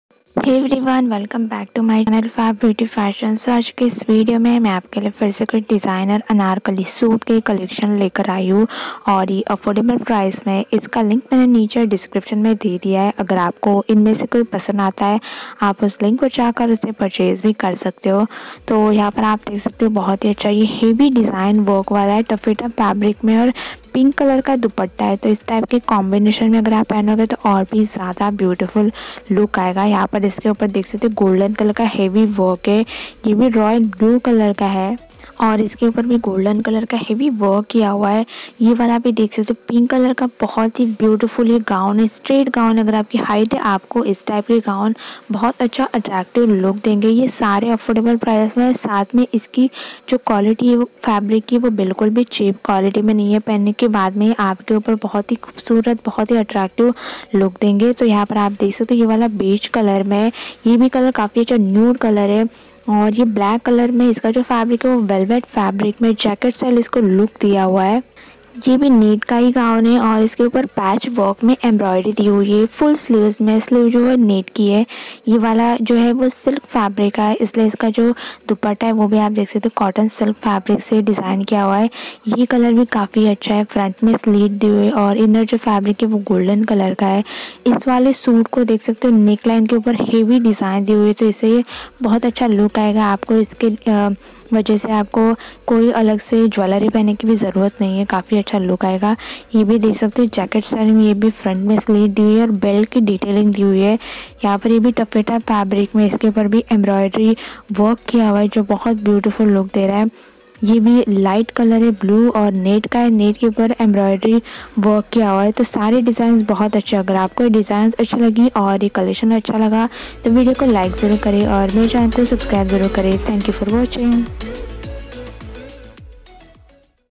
Designer Anarkali Suit With Dupatta Rs.80 Only // Anarkali Gown In Cheapest Rate
Hii Guys,

This are beautiful anarkali gown / party wear gown designs 2020 just for rs.80 I hope you like this collection.